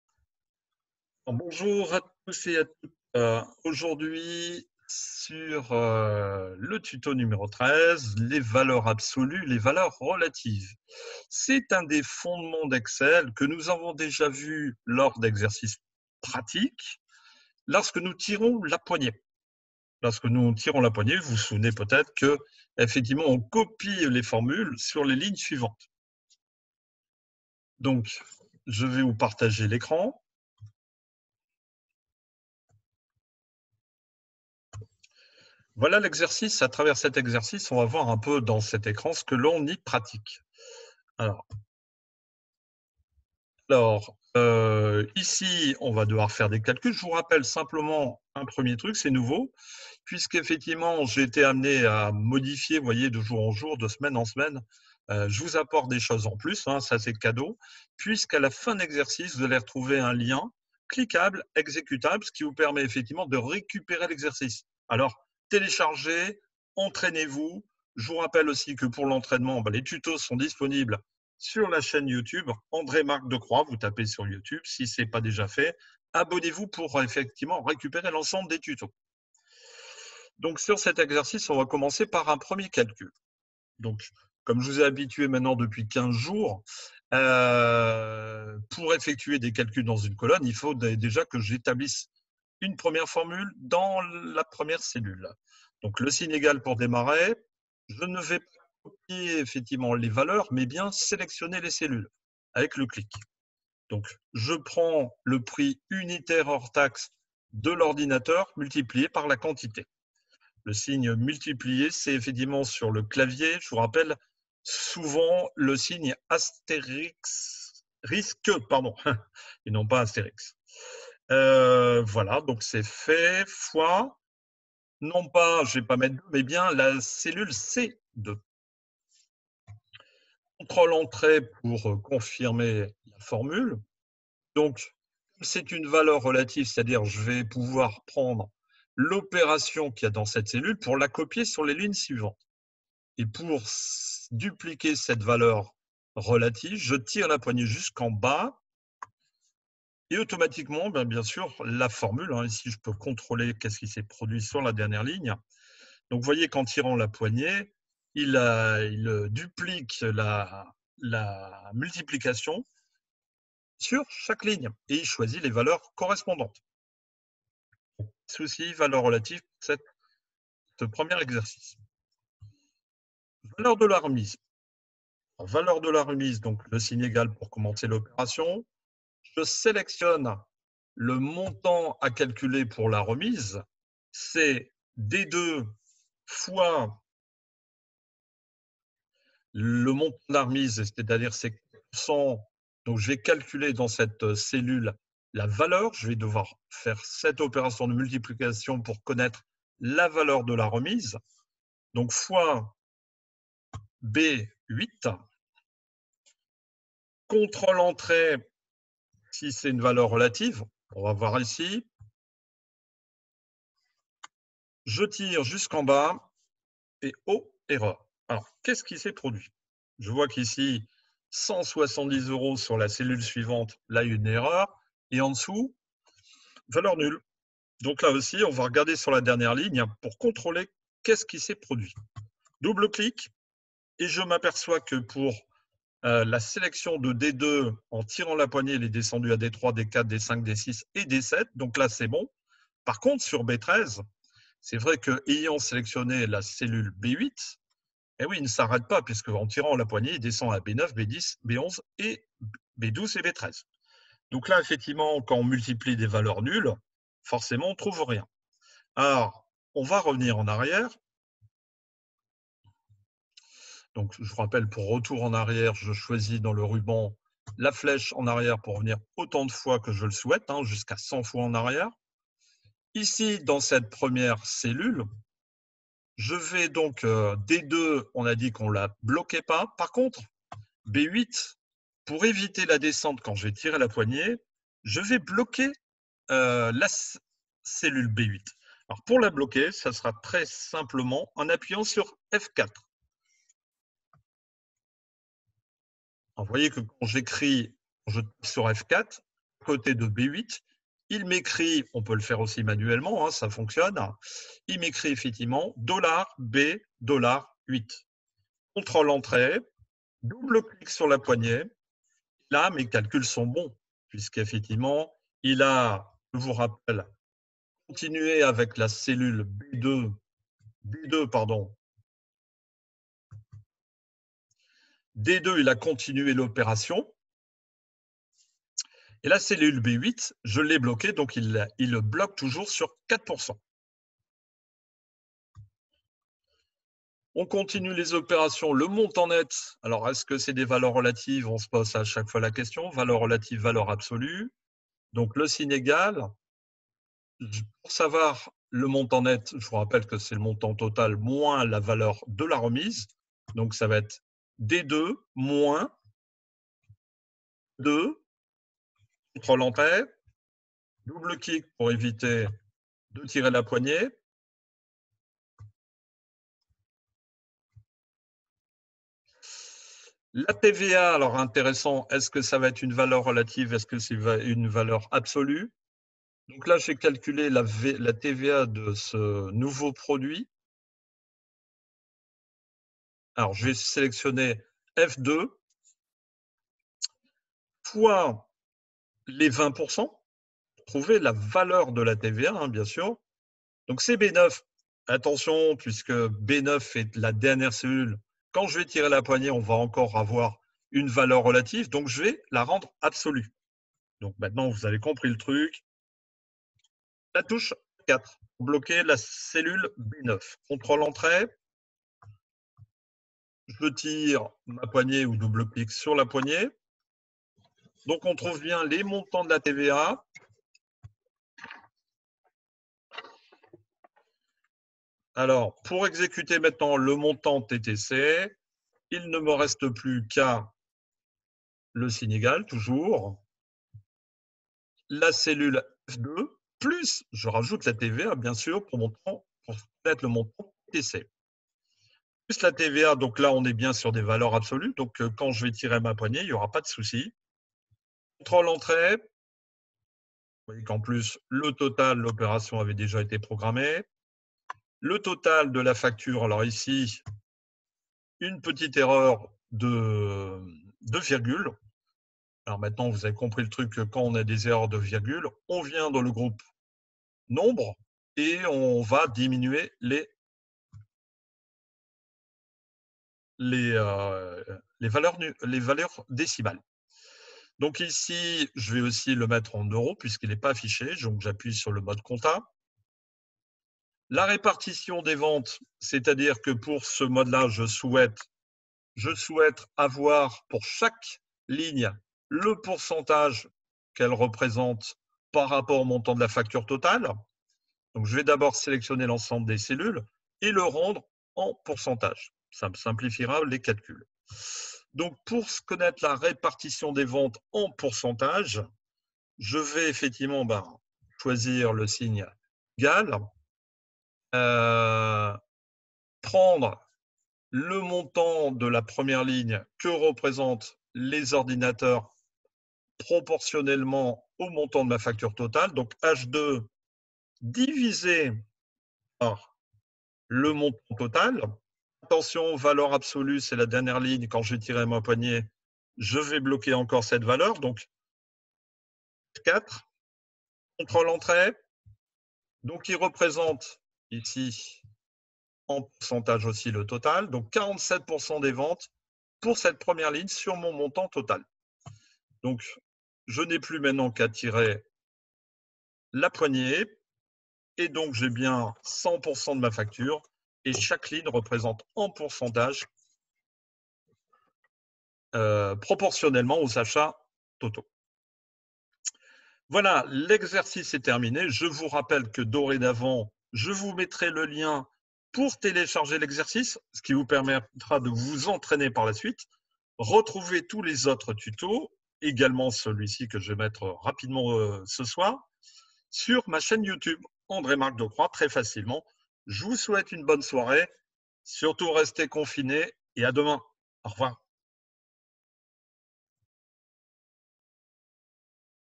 TUTO N° 13 LES VALEURS RELATIVES ET LES VALEURS ABSOLUES
A travers un exercice de facturation , nous allons aborder un des fondements  d'EXCEL .